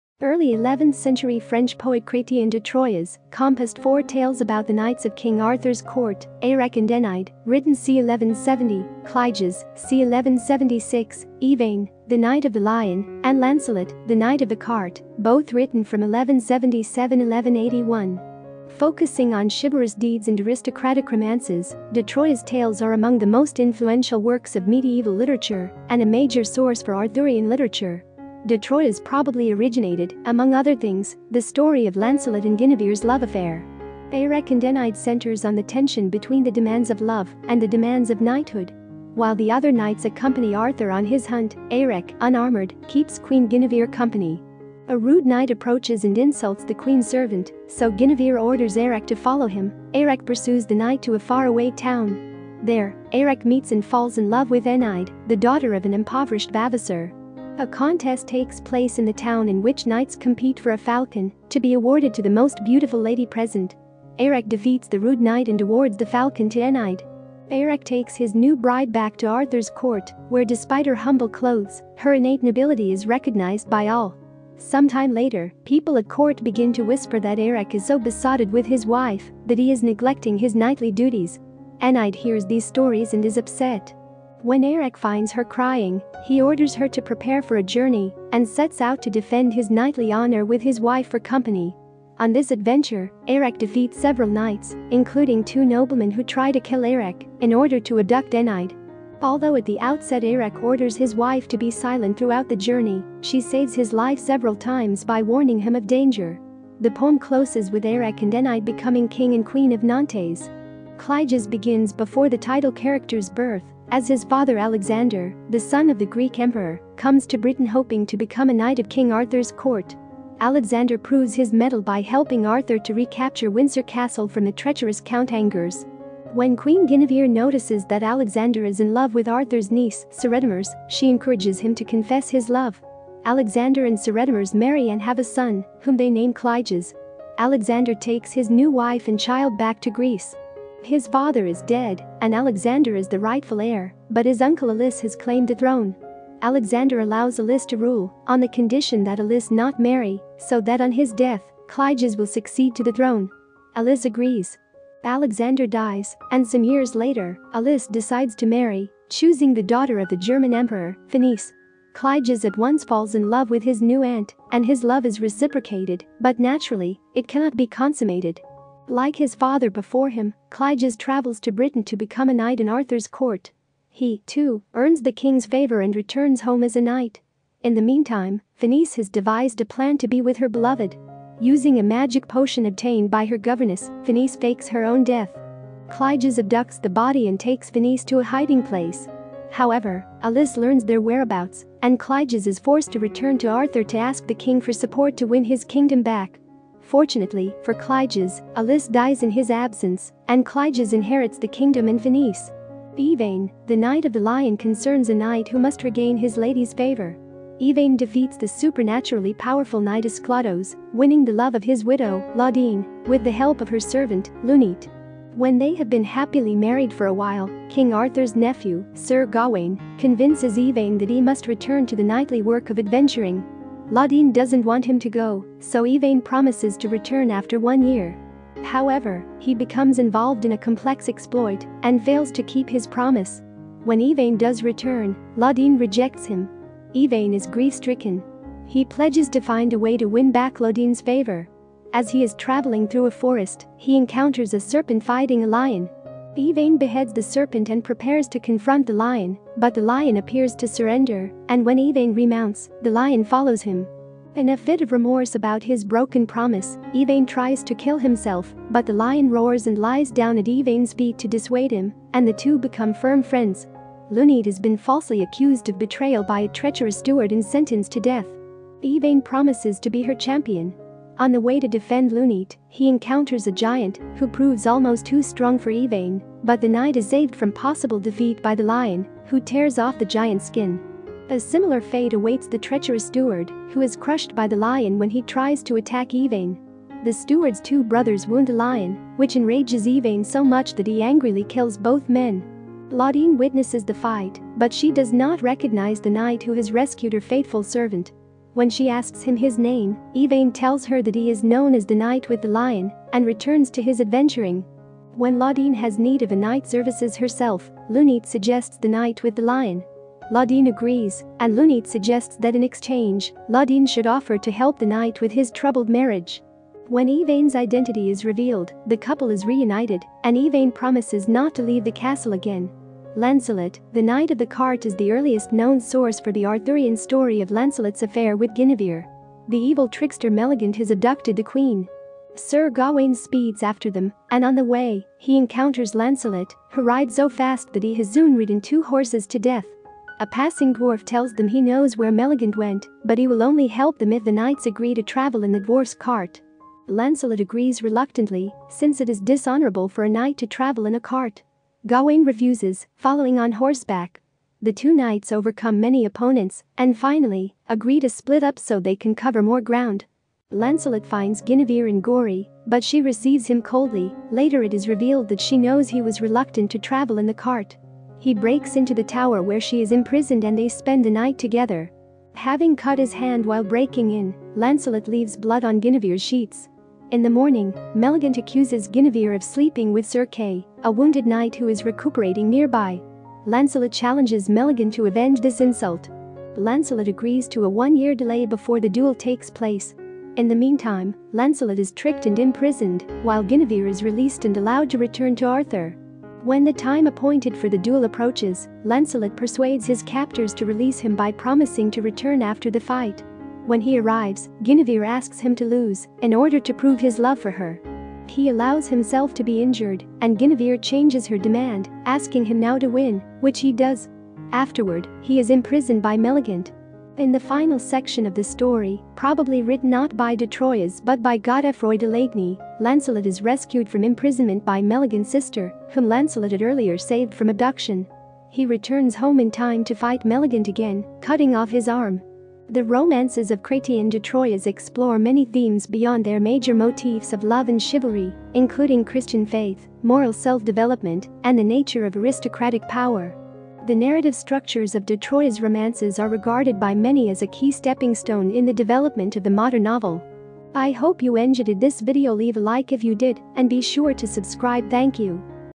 Plot summary, “Arthurian Romances” by Chrétien De Troyes in 7 Minutes - Book Review
Early eleventh-century French poet Chrétien de Troyes composed four tales about the knights of King Arthur’s court: Érec and Énide (written c. 1170), Cligès (c.1176), Yvain, the Knight of the Lion, and Lancelot, the Knight of the Cart (both written from 1177–1181).